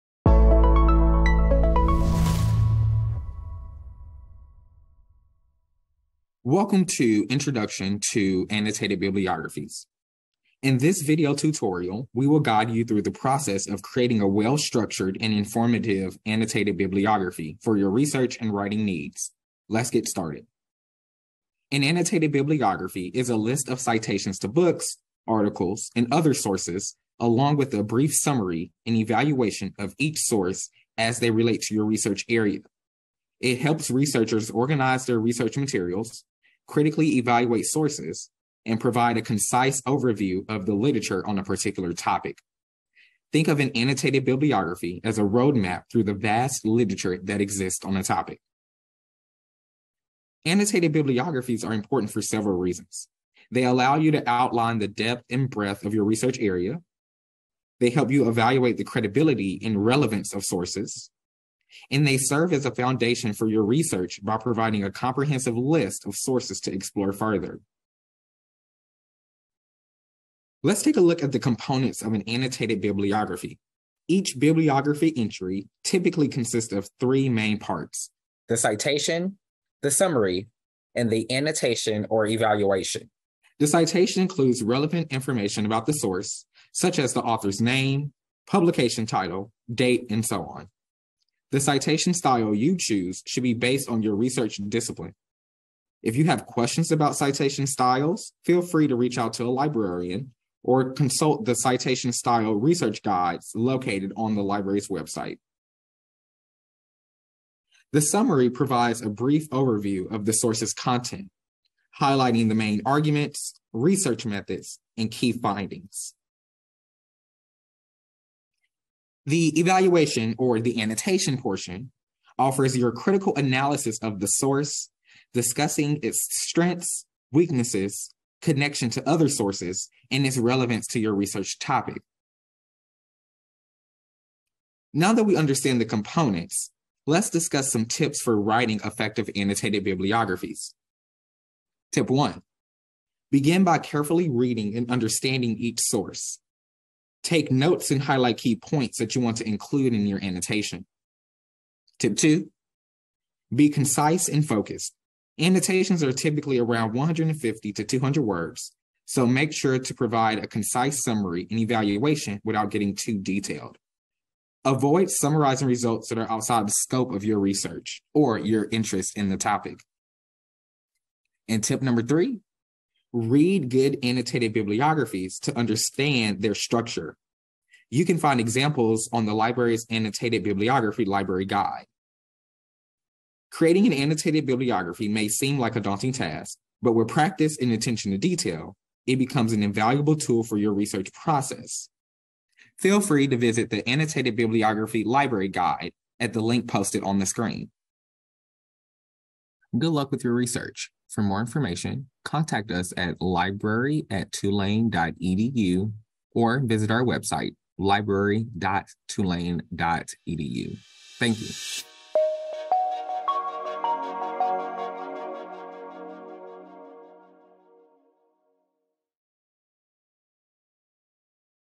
Introduction to Annotated Bibliographies - Tutorial
This tutorial will guide you through the process of creating a well-structured and informative annotated bibliography for your research and writing needs.

Tutorial created by Kay P Maye, Scholarly Engagement Librarian, Social Sciences and Data

Additional editing by Alan Velasquez